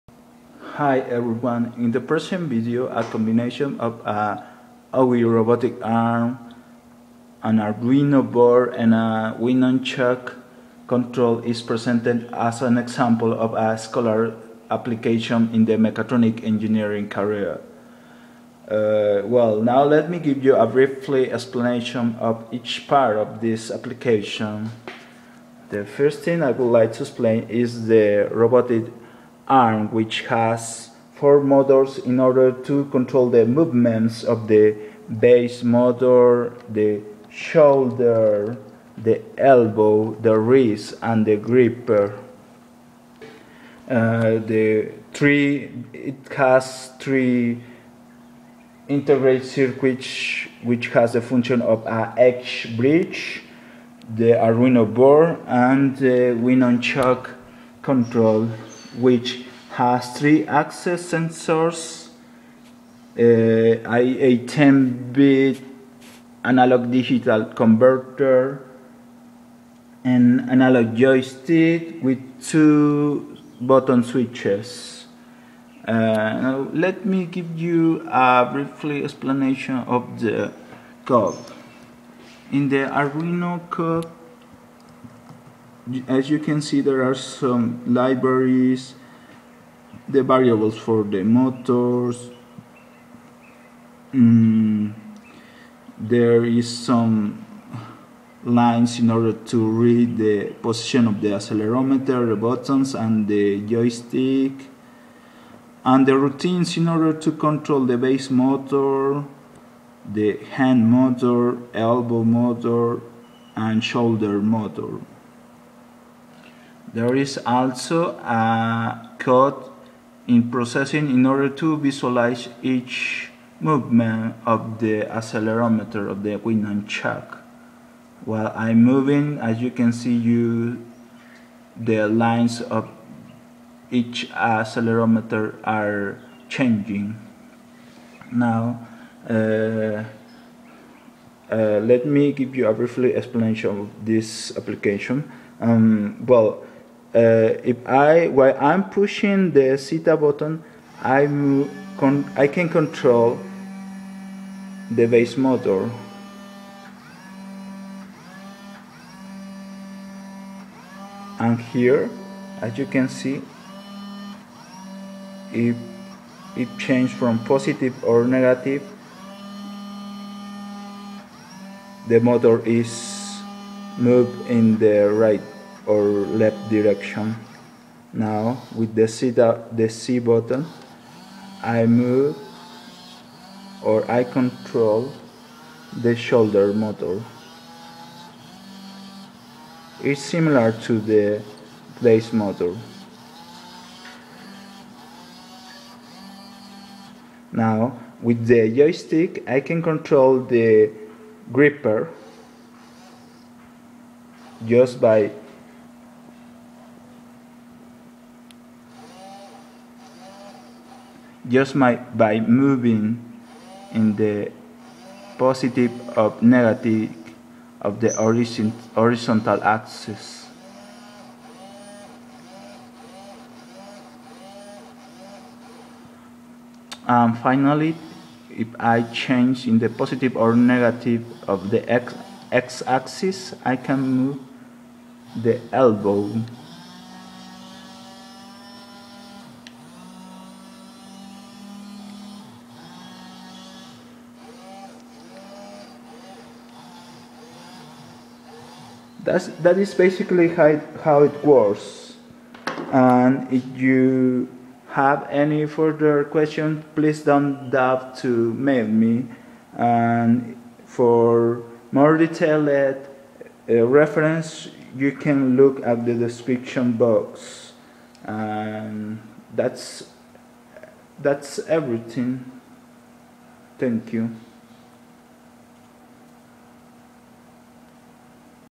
OWI 535 Robotic Arm control by Using an Arduino board and a Wii-nunchuk Controller
The OWI 535 Robotic Arm control was developed by using an arduino board, three L293 H-bridges [1], and a wii.nunchuk [2]. You are able to download the code for Arduino and Processing respectfully from the following links:

As a future work each joint is going to have a potentiometer to measure its position for "closed loop" feedback control [3,4].